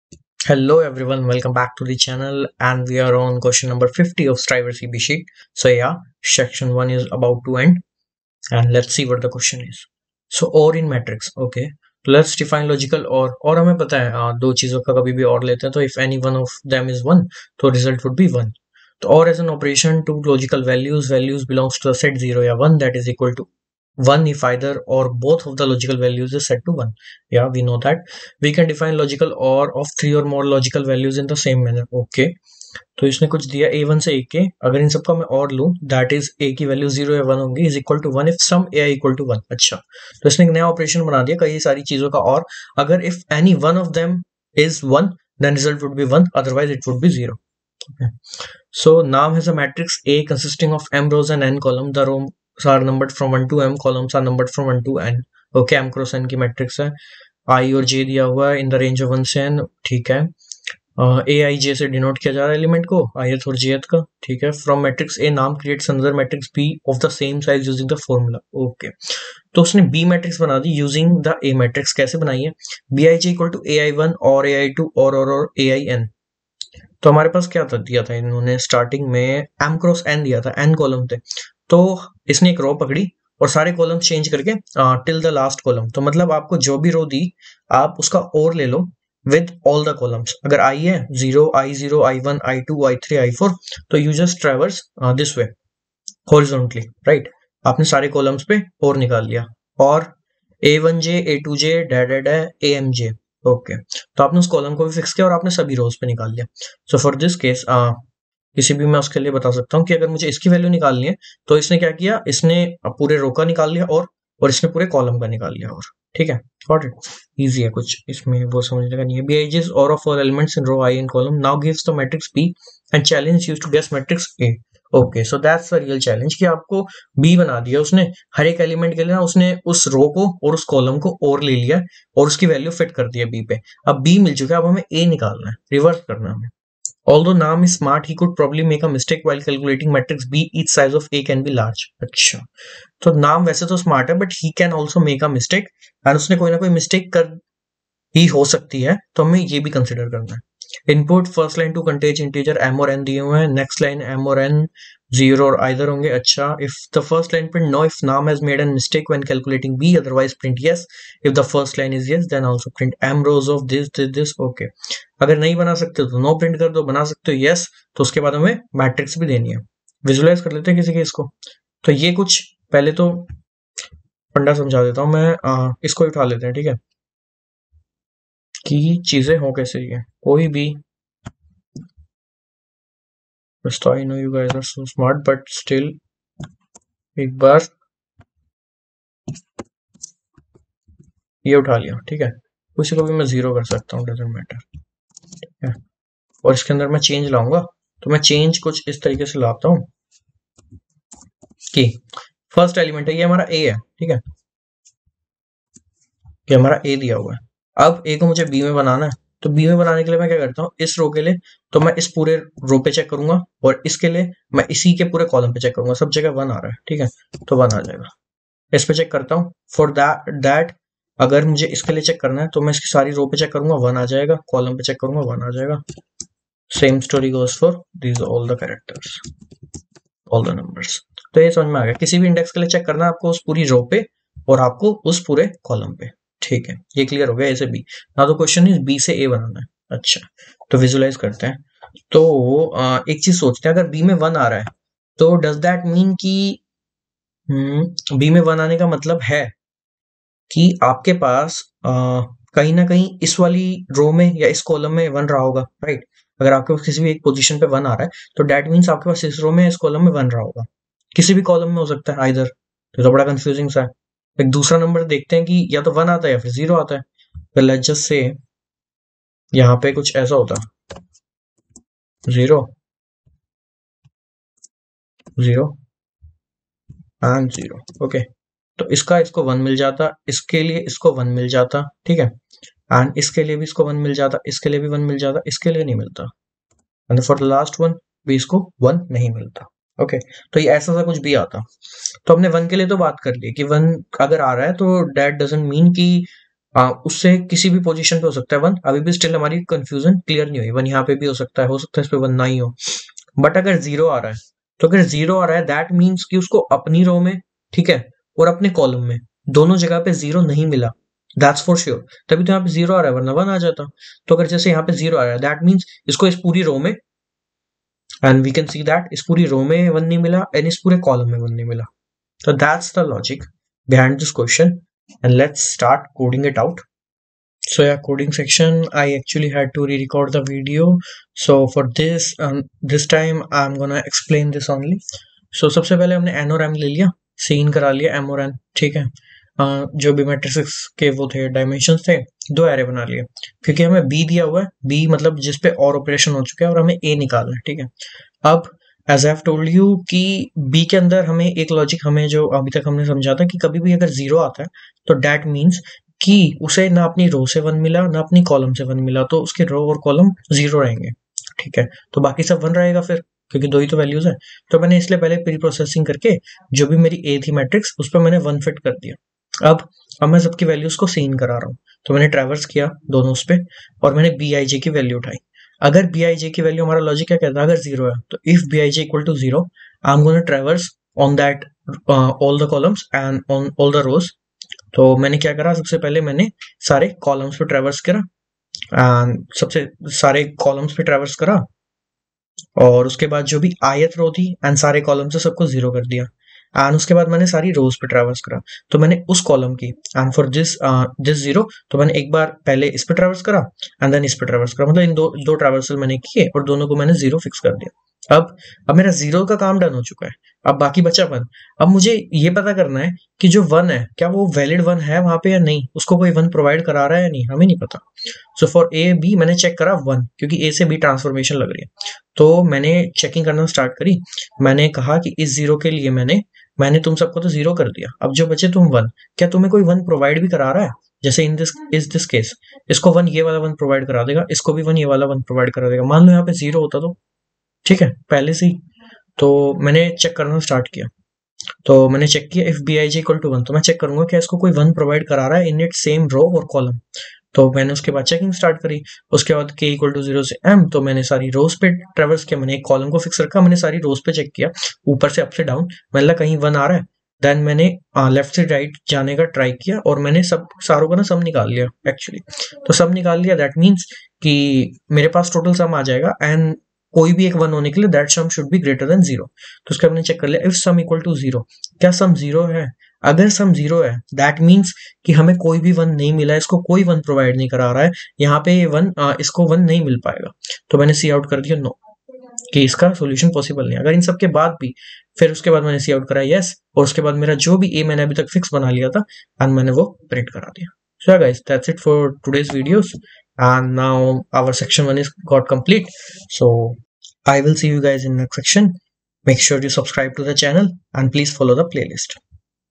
OR in Matrix | Striver CP Sheet | Codeforces | Competitive Programming
This is me doing Striver CP Sheet consistently. Make sure you check all other videos and build your competitive skills :)